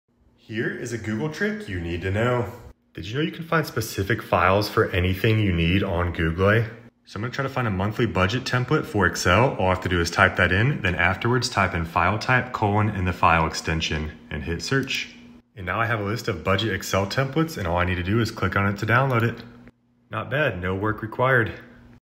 This Google Search Trick is a Lifesaver!
Use this Google Search query to find files for anything you may need!